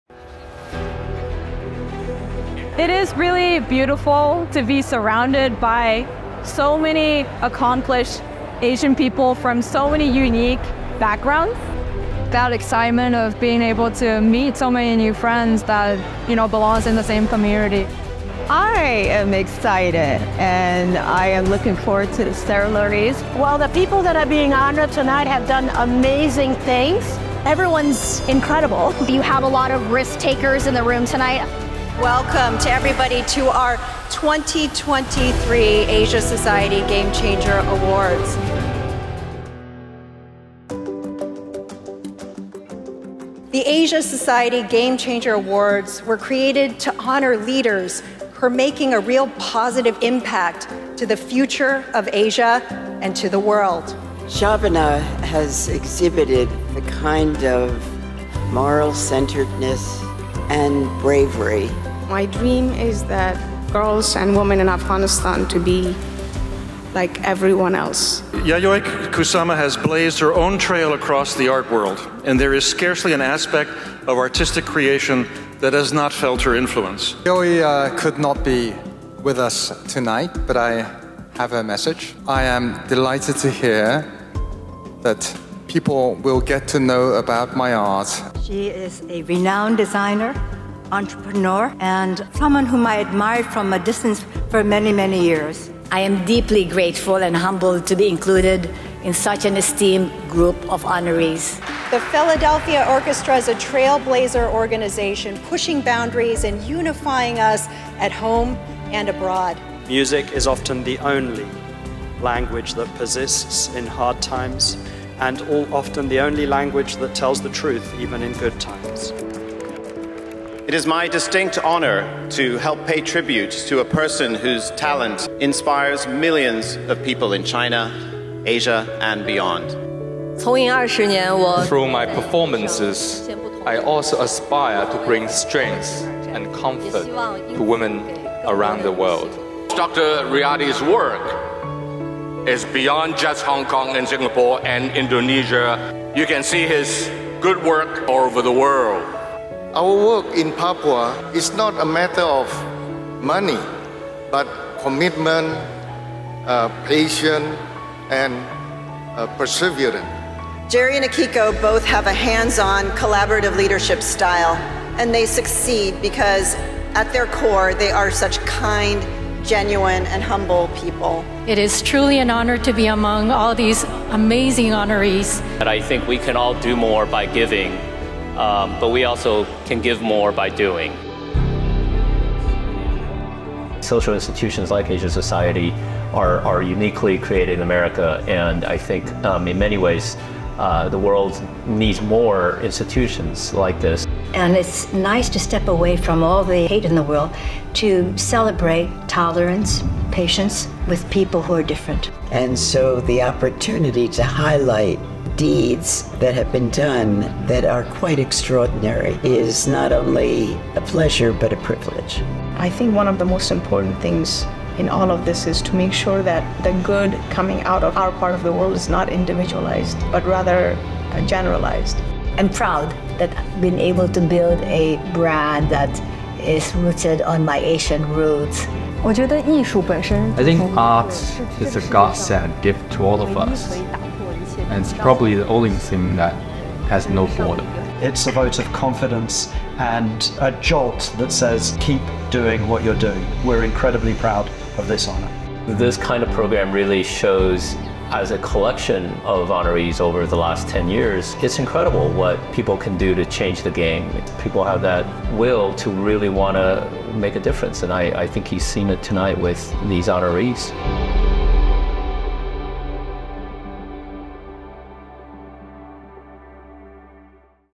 2023 Asia Game Changer Awards Highlights
NEW YORK, October 27, 2023 — The Asia Game Changer Awards recognize individuals and groups whose actions have profoundly strengthened the bonds between Asia and the world. In 2023, we celebrated our 10th Anniversary All Star Asia Game Changer Awards Ceremony and Gala.  (4 min., 28 sec.)